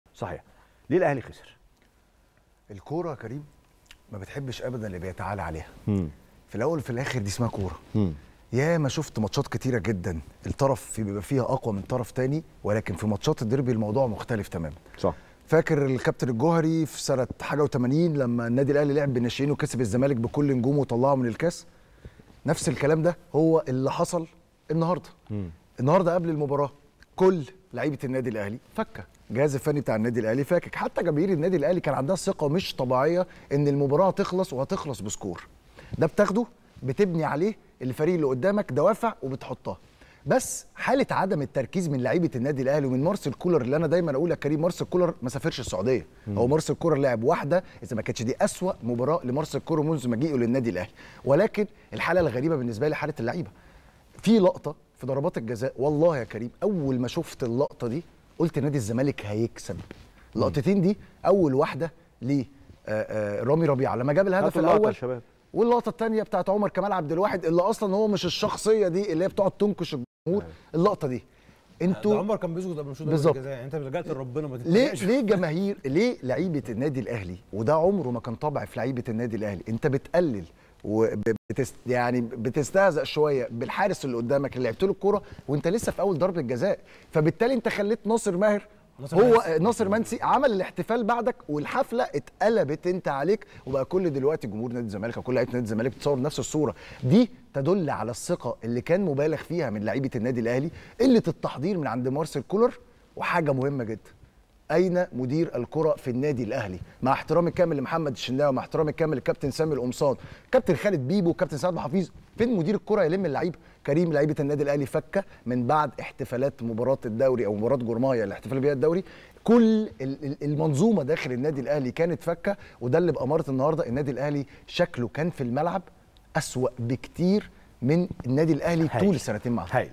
مارسيل كولر مسافرش السعودية"🚨🚨محمود شاكر هيقولك لية الأهلي خسر النهاردة أمام الزمالك 💣👇🏻
"الكرة مش بتحب اللي بيتعالي عليها .. مارسيل كولر مسافرش السعودية"🚨🚨محمود شاكر هيقولك لية الأهلي خسر النهاردة أمام الزمالك 💣👇🏻